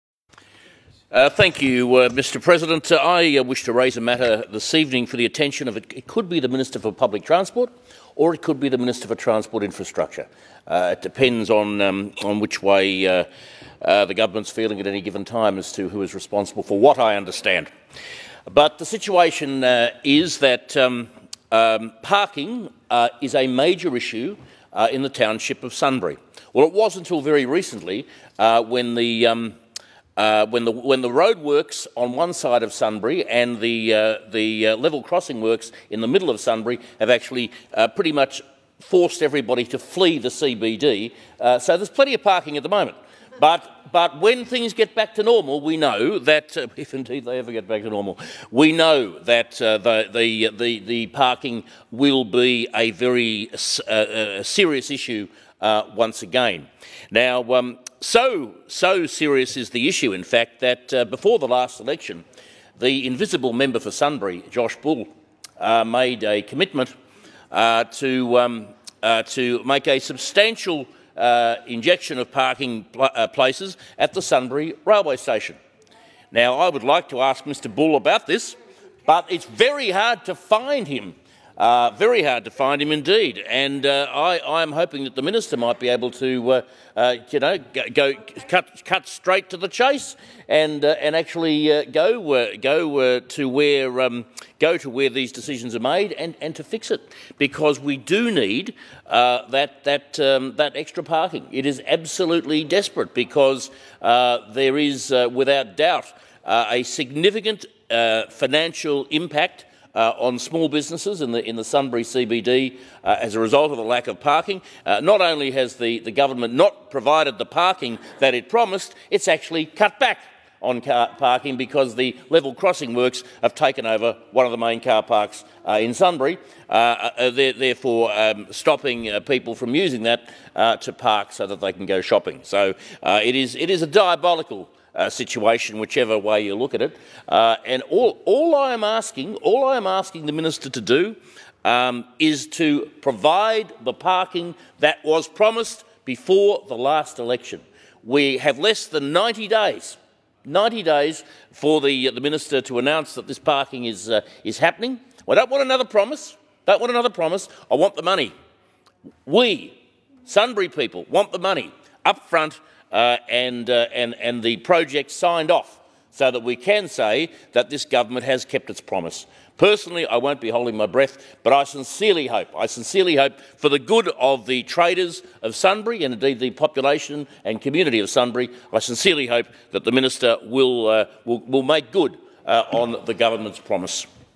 Where is The Invisible Man - Josh Bull?
Before the last election, the Andrews Government promised Sunbury more parking. Instead, it's cut car parking and given us traffic chaos-at the same time, Josh 'The Invisible Man' Bull, remains as silent as a tomb!